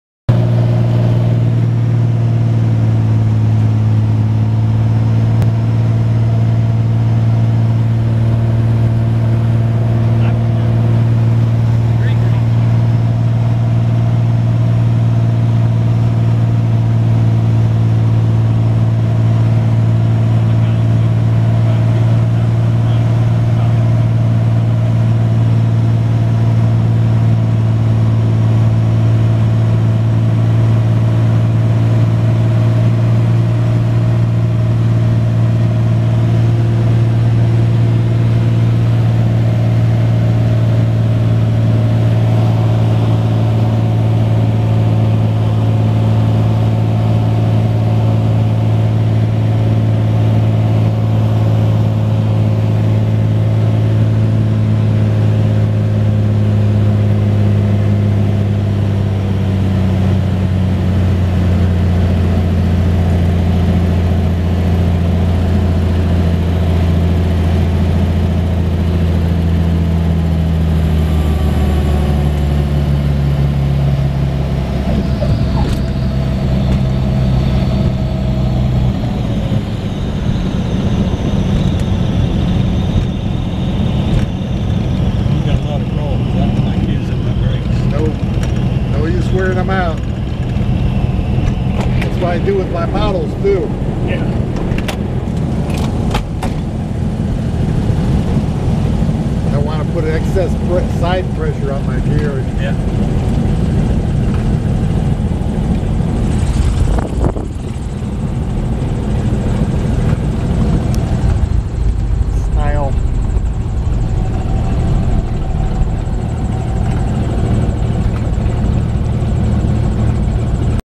Navion landing at Butler County airport, Ohio
Made by North American in 1947.